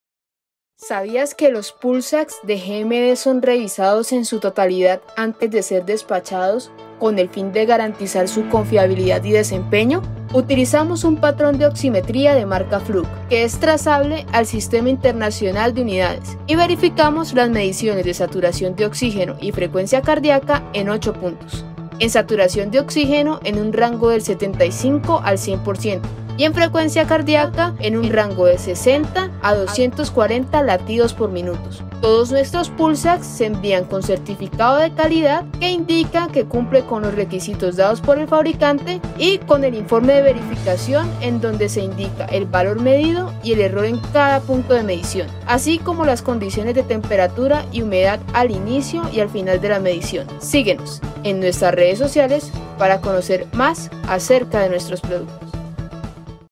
como se verifican los pulsioximetros de GMD
¿Sabías que todos los Pulsax GMD son verificados antes ser despachados? Conoce más del proceso en este video.

Recuerda que puedes comprar tus productos GMD, dando click en el siguiente enlace: